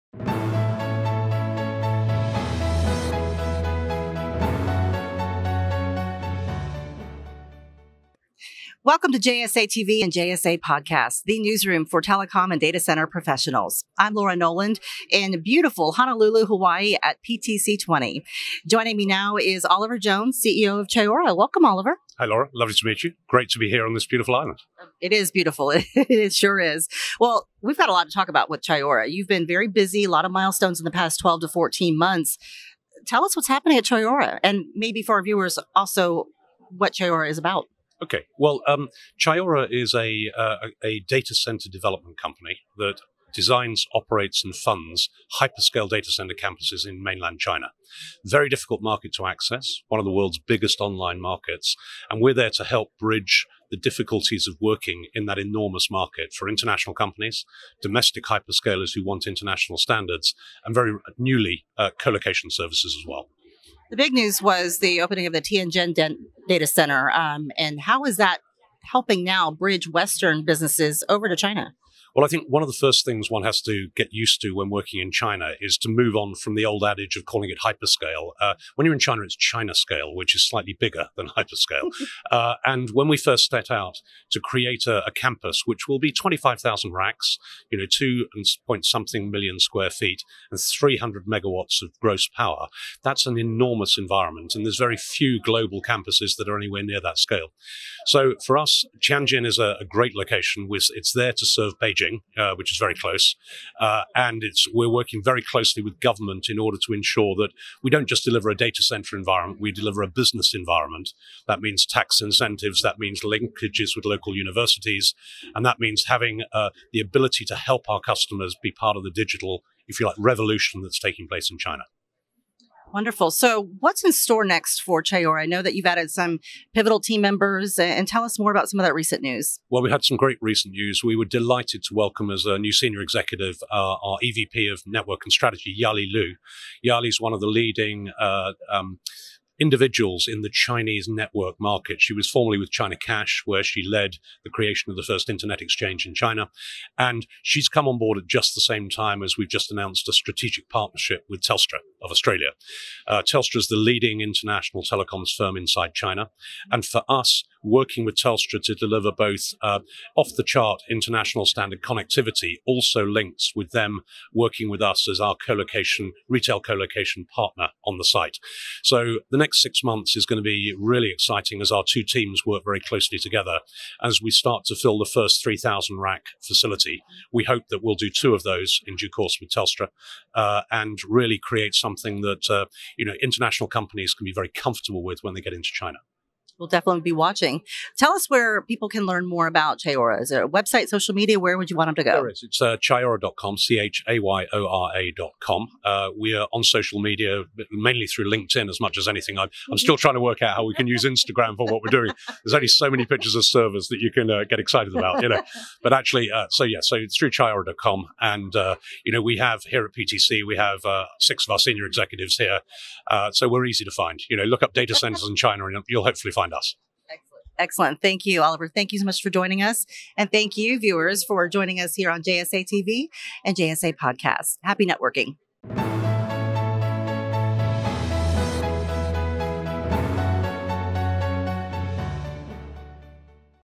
PTC'20: Chayora CEO on Helping Customers Be Part of the Digital Revolution Taking Place in China
Live from PTC'20, we are proud to welcome Mr. Oliver Jones, CEO of Chayora, a world-class infrastructure developer based in Hong Kong with a primary focus on building Data Centre platforms in China.

Chayora enables international online companies to effectively access the vast and rapidly developing Chinese market.

Chayora is developing a series of hyperscale, fully licensed data centre campuses in key locations across China: initially in Tianjin to serve the greater Beijing and northern provinces; and in Shanghai to serve the eastern provinces.